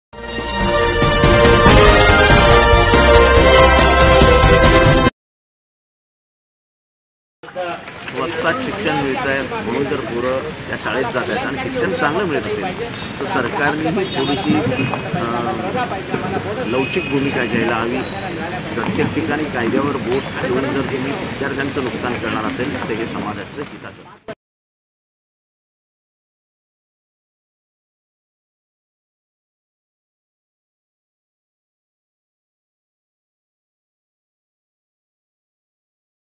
Politician Jitendra Ahwad on Unrecogised School of Maharashtra | Azad Maidan | Mumbai | 2018
16th/ October/ 2018

The RTE Act, 2009 passed by the Congress govt, as a Central Law requires schools to have a proper building with Facility for Classrooms, Library, Bathrooms, Staffrooms & Playgrounds. The act didn't include the 'Size' of the Building or the Class rooms.
     But, The Maharashtra Government (Congress) in year 2013 passed the Self Financed School Act and Made it Compulsory for new schools in Cities to have minimum 1 acre of Land. Most of the schools started after 2013 could not get recognition under the state law and were termed illegal.
   Such Schools are more than 50 across Maharashtra with Teachers Staff of 1200+ & More than 18000+ Students. According, to 'Maharashtra Unrecognized Schools Association' this Schools should be Legal & grant Permission from Govt, of Maaharashtra rather than shutting them down & loss of Small Childeren in Education & Unemployment of Staff...

Thus, The Above Information is provided by the, 'Maharashtra Unrecognized Schools Association' in thier Protest program at Azad Maidan | Mumbai | 2018